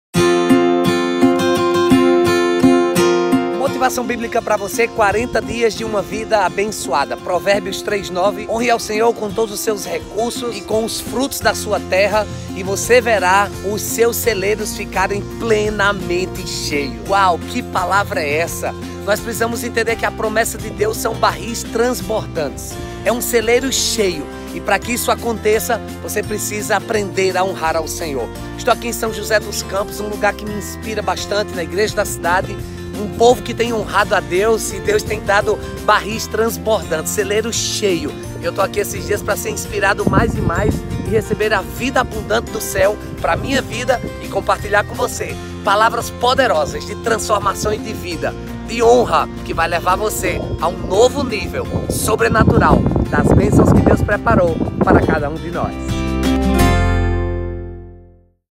Honre ao Senhor
Se liga nesse vídeo com motivações bíblicas na campanha 40 dias de uma vida abençoada ministrado pelo Bispo Márcio Meira da Igreja Anglicana djp.anglicananobrasil.com e hoje pastoreia a anglicanacomunhao.com